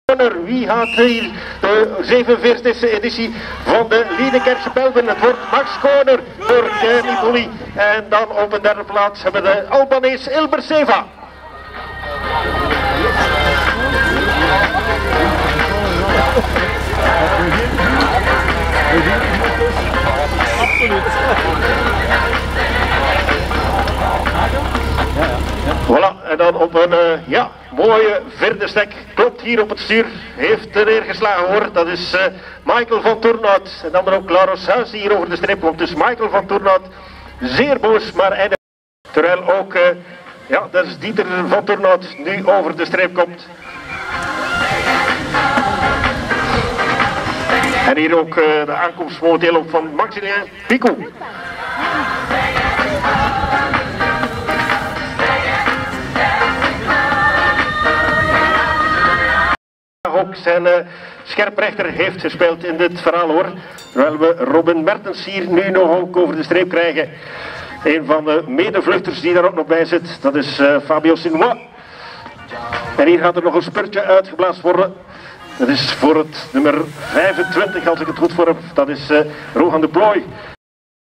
Liedekerkse pijl
Liedekerkse pijl - aankomst eerste 22 renners - Han op plaats 20